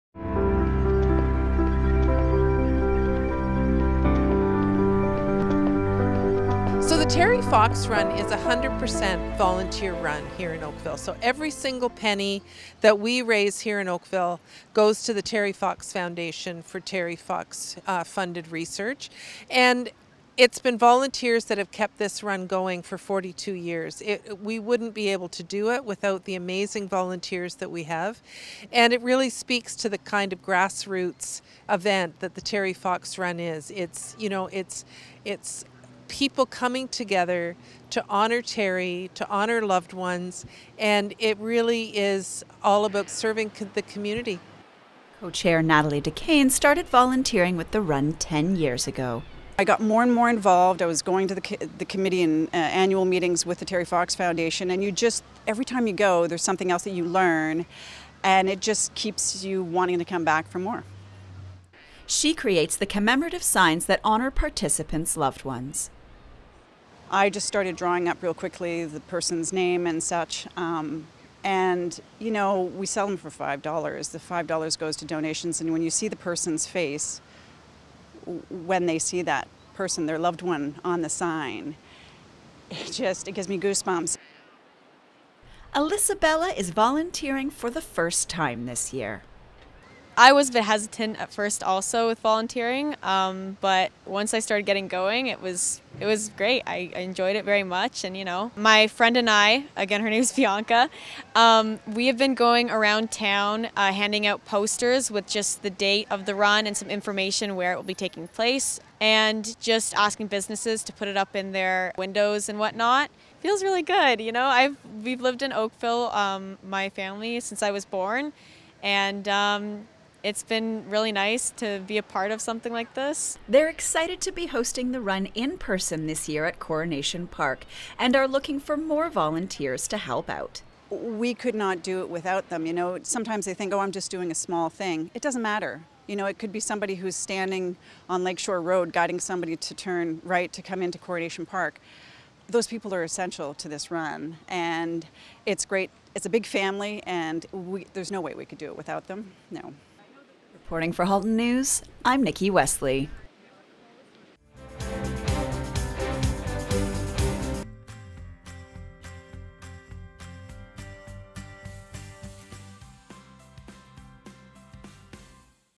Oakville Terry Fox Run
Now in its 42 year, the Terry Fox Marathon of Hope wouldn't exist if it wasn't for the never ending work of volunteers and it all started with Terry volunteering his time to raise awareness. Nikki Wesley speaks with the Oakville Terry Fox Co-Chairs about the importance of volunteers as they return to an in person event.